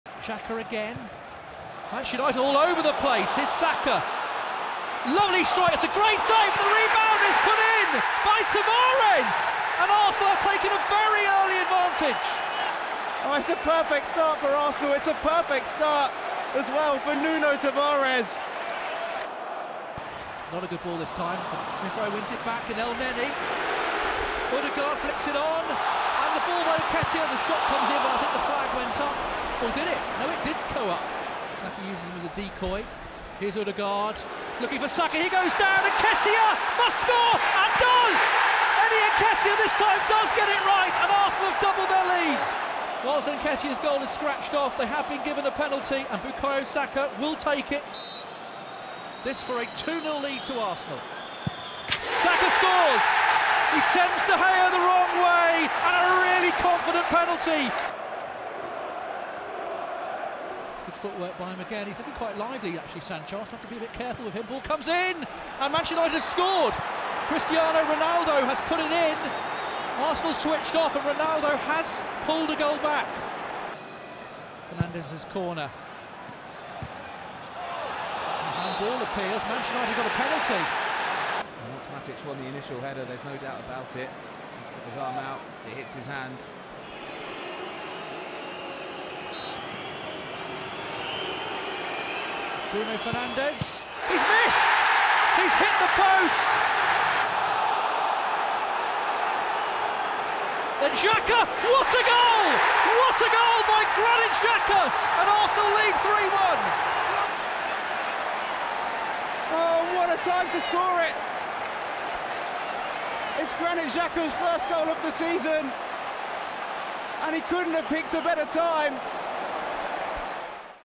HIGHLIGHTS | Arsenal vs Manchester United (3-1) | Premier League | Tavares, Saka, Xhaka-boom!
We moved back into the top four thanks to a pulsating victory over Manchester United.

It was a highly charged, incident-packed encounter from start to finish. Nuno Tavares and Bukayo Saka fired us into a two-goal lead inside half an hour, before United rallied to halve the deficit through Cristiano Ronaldo.

Bruno Fernandes then missed a penalty before Granit Xhaka sealed the points with a superb 25-yard drive.

These lunchtime kick offs can often have a sedate atmosphere early on. Not this one. The crowd were up for it from the kick off, and the team responded immediately.

There were just two minutes on the clock when Tavares gave us the lead. Xhaka’s deep cross was missed by everyone in the United defence and found its way though to Saka. His shot was parried by David de Gea but Tavares was in the right place at the far post to tuck home.

It was the full back’s first goal for the club since joining from Benfica last summer.

United came back. Anthony Elanga had a low shot well saved by Aaron Ramsdale before Gabriel put in a great block after Ramsdale had presented the ball to Fernandes.

Diogo Dalot was next to go close for the visitors, curling a shot from 20 yards over the keeper and against the crossbar.

Chances were coming at both ends, and Eddie Nketiah had a great chance after being slid through on goal by Martin Odegaard but his shot was straight at de Gea.

Moments later Nketiah thought he had beaten the keeper, only for the goal to be ruled out by VAR for a very close offside call. However there was a foul on Saka in the build up, and a spot kick was awarded instead.

For the second time in a week the England man stepped up and netted from the penalty spot.

The two-goal advantage didn’t last long. United had been a threat throughout the opening half hour, and pulled a goal back through Cristiano Ronaldo, who stole in at the far post to fire home a looping cross. Ramsdale’s moment of hesitation proved costly.

The first half was action-packed, and had flown by. Odegaard had another shot tipped away in injury-time – one of six attempts on target in the opening 45 minutes for us.

Tavares should have hit the target soon after the interval. He found space inside the box, beat his man superbly, but blazed a shot high and wide.

Tavares’s eventful afternoon continued when he gave away a penalty for handball. Fernandes stepped up, but his shot hit the post and went behind for a goal kick.

It was a big let off, and moments later there was another when Ronaldo’s goal was ruled out for offside by the barest of margins.

Ramsdale came up with a huge save at his near post to turn Dalot’s shot onto the woodwork.

We were living dangerously, but Granit Xhaka’s fierce drive relieved the pressure.

He picked up the ball five yards outside the box, shifted it on to his trusty left foot, and arrowed his shot beyond the despairing de Gea just inside the post.

The Emirates erupted. With the two-goal cushion restored, we closed out the game to earn a second big win in the space of four days.

Teams:

Arsenal: Ramsdale, Cedric, White, Gabriel, Tavares; Elneny, Xhaka;, Saka, Odegaard, Smith Rowe,  Nketiah.
Substitutes: Leno, Tomiyasu, Holding, Lokonga, Azeez, Swanson, Pepe, Martinelli, Lacazette

Manchester United: De Gea, Dalot, Lindelof, Varane, Alex Telles,  McTominay, Matic,  Elanga, Fernandes, Sancho, Ronaldo.
Substitutes  Henderson, Wan-Bissaka, Bailly, Jones, Maguire, Mata, Lingard, Rashford, Garnacho.